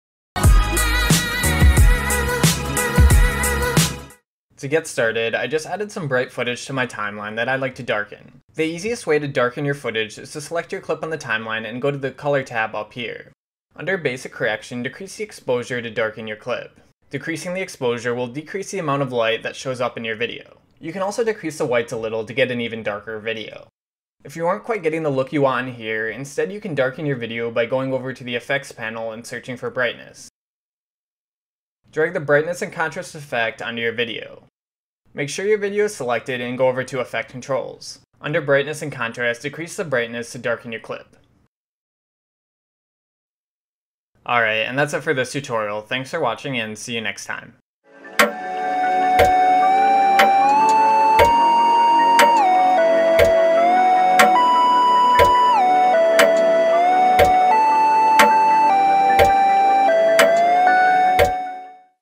Premiere Pro : How to Darken Video Footage
A tutorial on how to darken a video clip in premiere pro cc.